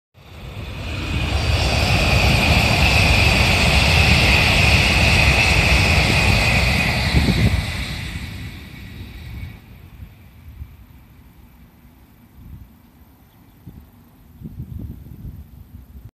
N700系新幹線(下り)通過
N700系新幹線が通過してゆくところを撮影してみました